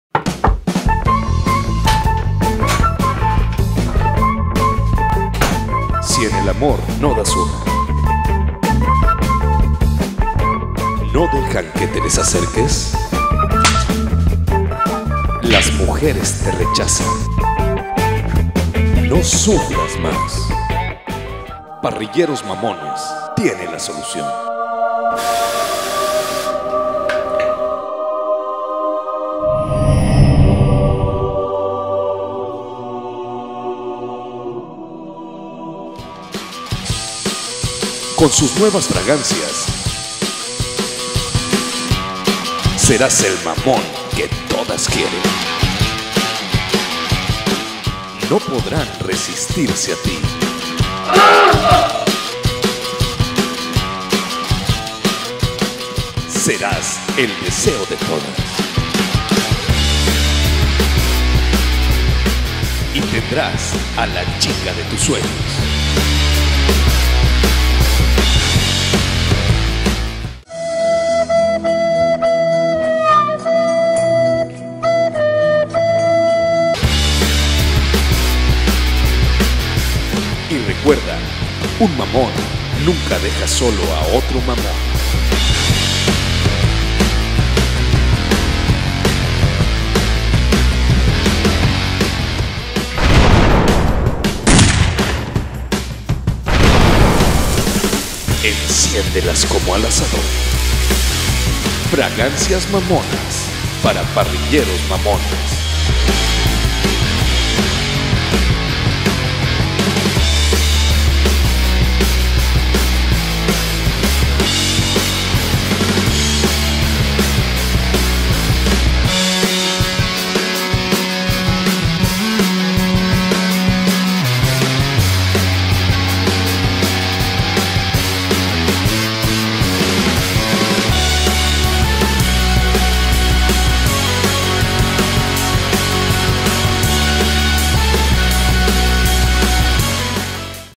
Fragancias Mamonas 🔥👌🍆💦🔥
¡YA NO MÁS! Les dejamos un tip muy mamón, para parrilleros muy mamones. Etiqueta a tu amigo el que no tiene novia, tiene Tinder y que necesita urgentemente de esto. 🔥🔥👌🍆💦🔥🔥